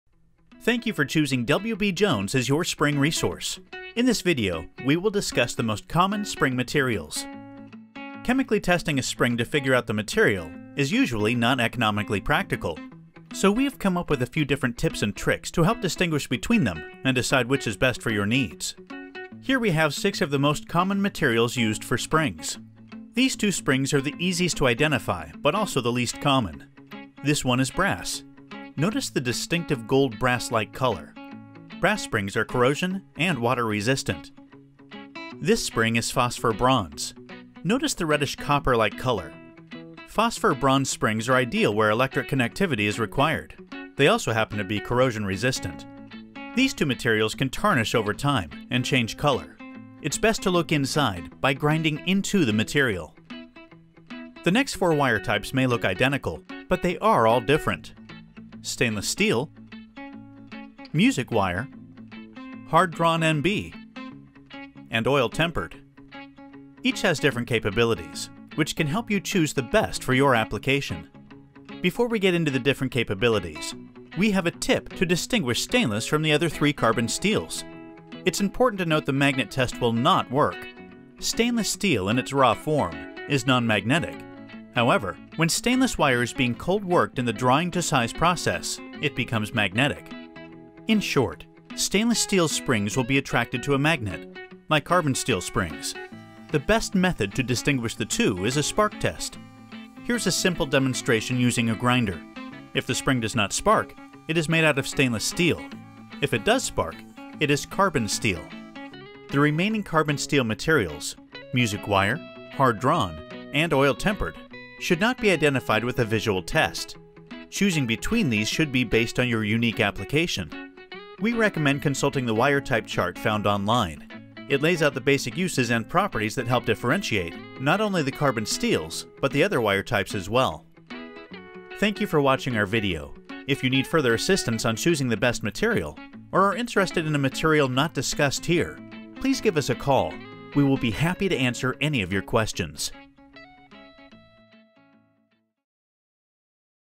Understanding Different Spring Materials
To find the perfect spring for your industrial needs, understanding different spring materials is key. This video aims to cover common materials used in springs, different wire types and some helpful tips and tricks.
*** FULL VIDEO TRANSCRIPT BELOW ***
Thank you for choosing W.B. Jones as your spring resource. In this video, we will discuss the most common spring materials chemically testing a spring to figure out the material is usually not economically practical. So we have come up with a few different tips and tricks to help distinguish between them and decide which is best for your needs. Here we have six of the most common materials used for springs.

These two springs are the easiest to identify, but also the least common. This one is brass. Notice the distinctive gold brass like color. Brass springs are corrosion and water resistant. This spring is phosphor bronze. Notice the reddish copper like color. Phosphor bronze springs are ideal where electric connectivity is required. They also happen to be corrosion resistant. These two materials can tarnish over time and change color.

It's best to look inside by grinding into the material. The next four wire types may look identical, but they are all different. Stainless steel music wire, hard drawn, NB and oil tempered. Each has different capabilities which can help you choose the best for your application. Before we get into the different capabilities, we have a tip to distinguish stainless from the other three carbon steels.

It's important to note the magnet test will not work. Stainless steel in its raw form is non-magnetic. However, when stainless wire is being cold worked in the drying to size process, it becomes magnetic. In short, stainless steel springs will be attracted to a magnet like carbon steel springs. The best method to distinguish the two is a spark test.

Here's a simple demonstration using a grinder. If the spring does not spark, it is made out of stainless steel. If it does spark, it is carbon steel. The remaining carbon steel materials, music wire, hard drawn and oil tempered should not be identified with a visual test. Choosing between these should be based on your unique application. We recommend consulting the wire type chart found online.

It lays out the basic uses and properties that help differentiate not only the carbon steels but the other wire types as well. If you need further assistance on choosing the best material or are interested in the material not discussed here, please give us a call. We will be happy to answer any of your questions.